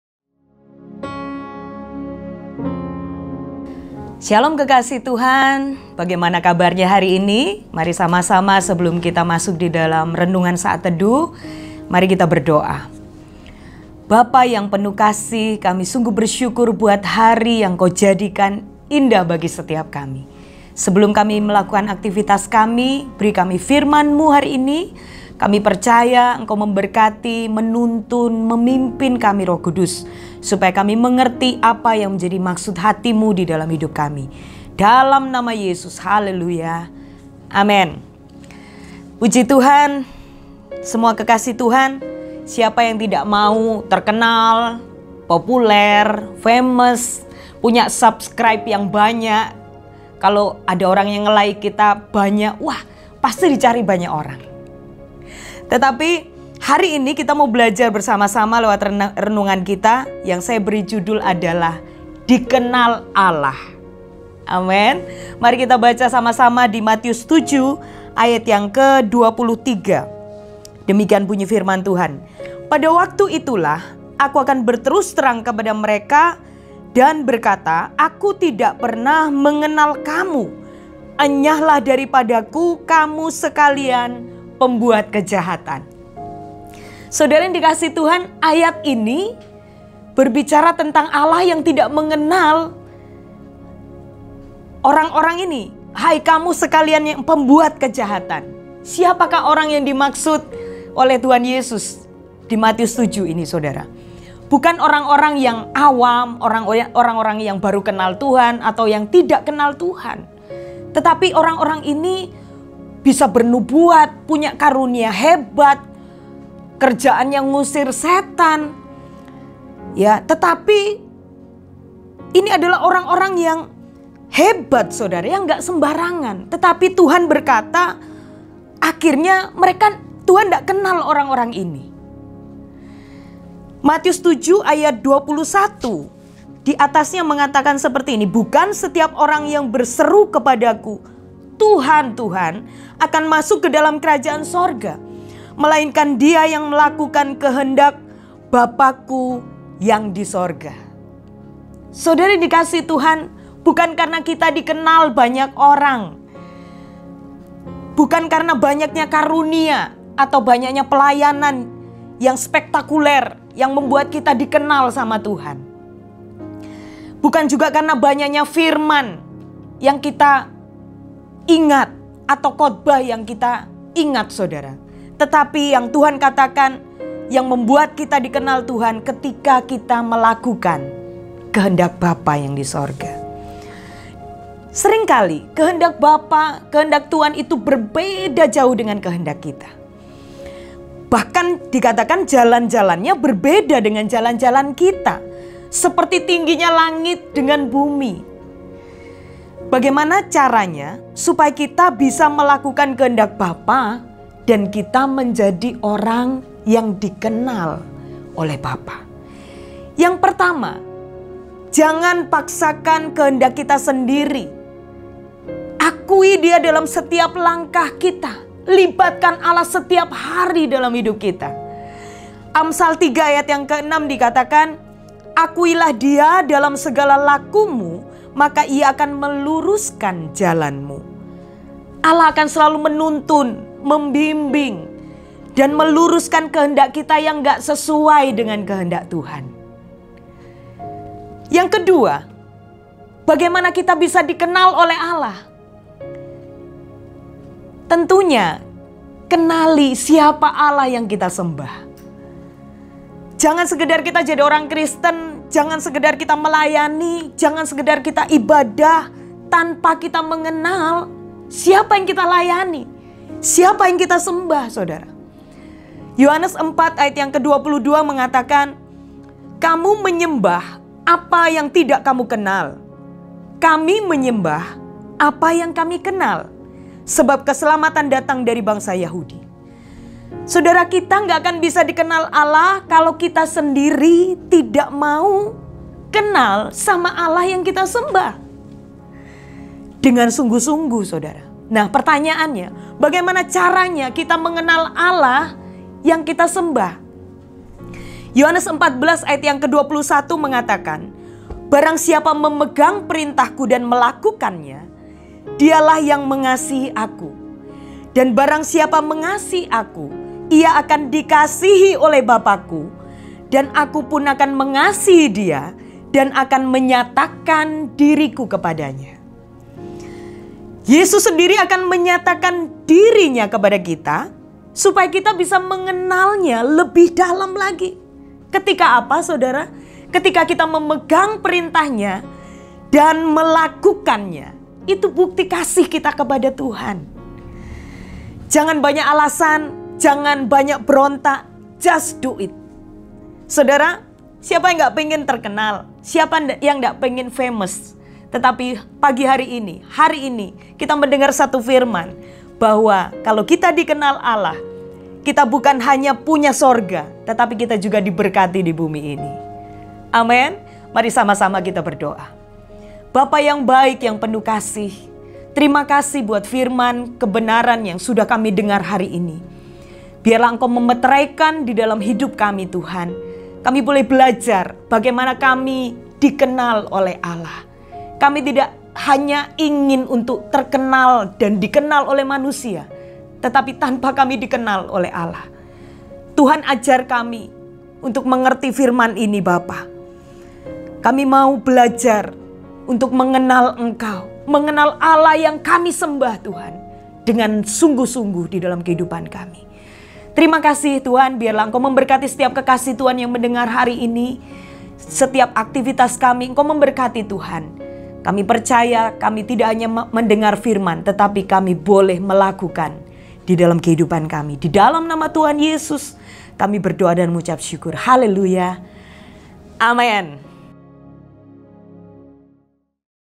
Saat Teduh "Dikenal Allah" (2 Juni 2020) | Ps Kezia Tantri
Bethany Miracle Center Sumurwelut Surabaya
Jl. Sumurwelut 01, Lakarsantri, Surabaya